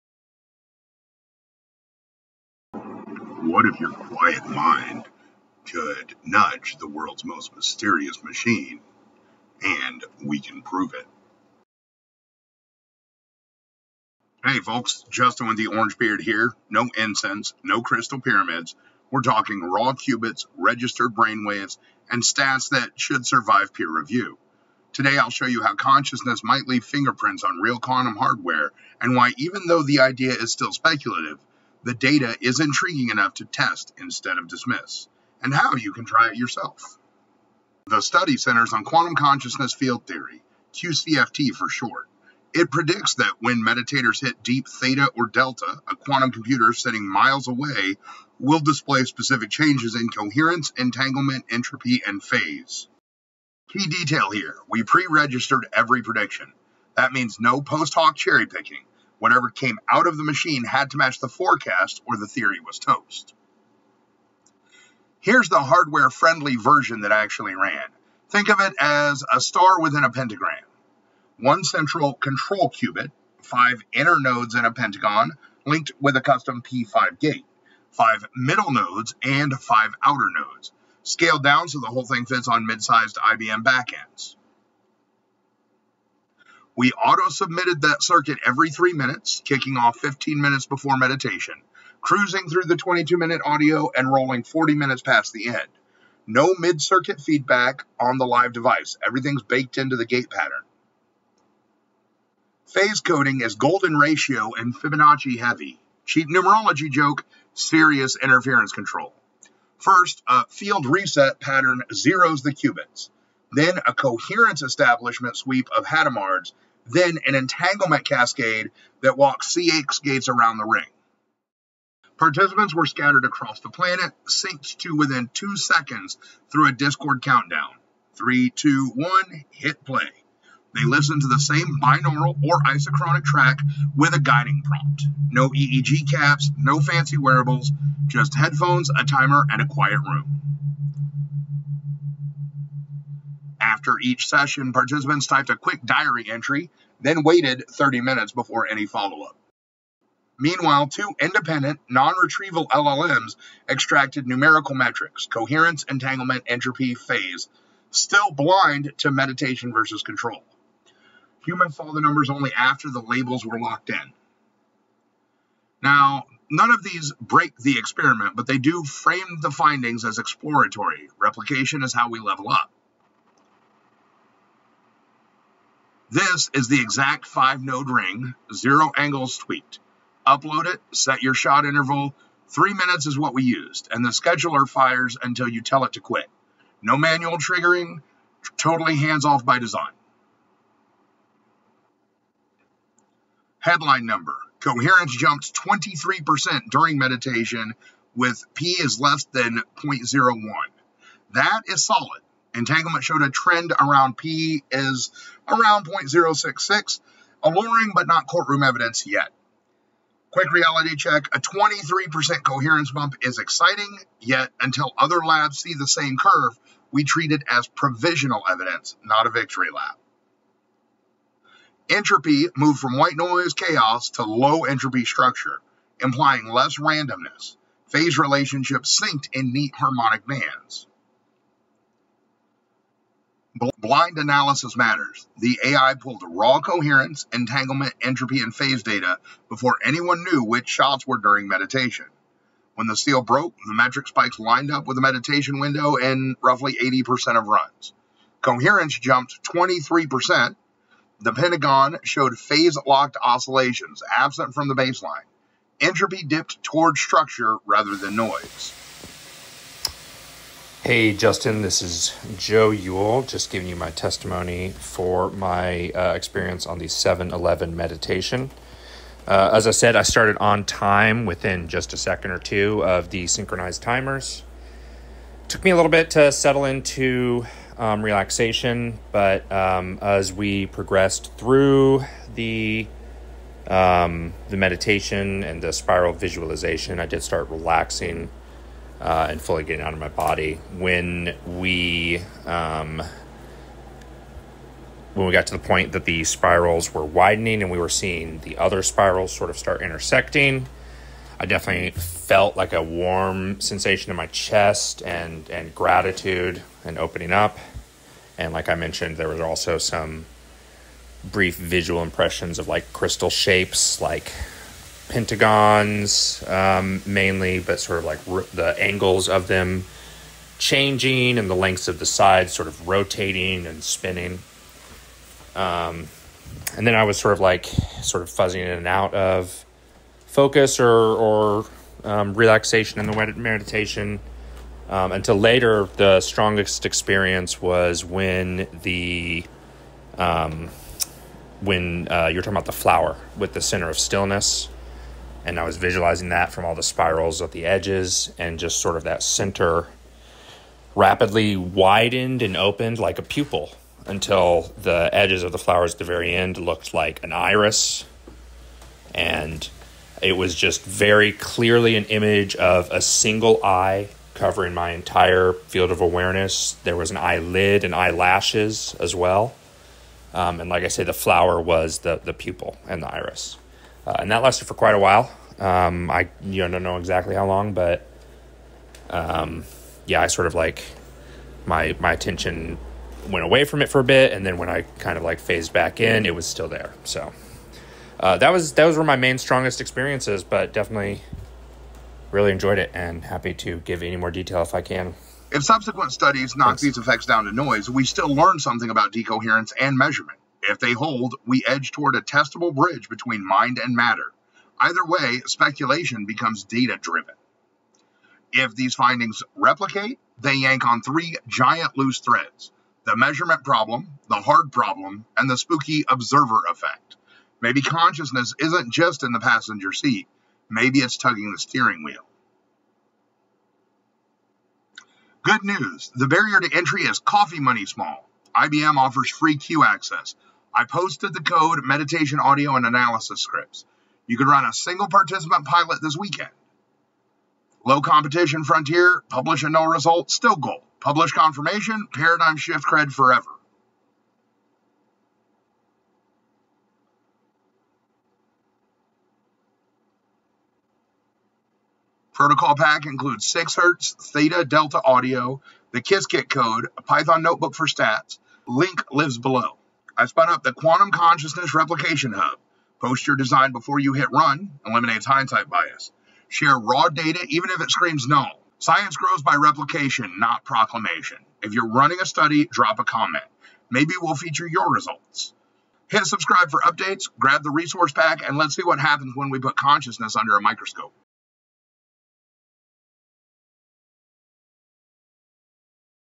What Insanity Happens When Mind Meets Quantum Tech?
Can Human Consciousness Influence Quantum Reality? We Put It to the Test.

Can your thoughts affect quantum systems?

In this groundbreaking experiment, we explore the intersection of quantum physics, neuroscience, and consciousness technology—putting human intention to the test on real quantum hardware. Using synchronized meditation, binaural beats, and custom quantum circuits, we measure changes in coherence, interference, and entanglement during altered states of awareness.

🔬 Over 7 months, 20+ participants across 70+ tests submitted quantum jobs in perfect sync with meditation states. The result? Statistically significant shifts that challenge what we thought we knew about the nature of reality.

🔗 Links & Resources
📁 Full Experiment Protocol

📊 Math Formalism, Source Code, Predictions
→ See full documentation in the GitHub above

The scientific protocol behind the experiment

How we used real IBM Quantum hardware (Torino backend)

Custom-designed Pentagonal Resonance Circuits

Brainwave-synchronized meditation audio

Real quantum data and decoded bitstrings

Initial results and what they could mean for science, technology, and consciousness

If validated independently, these results could revolutionize:

Quantum computing

Neuroscience

The philosophy of mind

The future of human-computer interaction
This is not metaphysics. This is math, code, and conscious coherence—put to the test.

👁‍🗨 You are not just an observer. You are the experiment.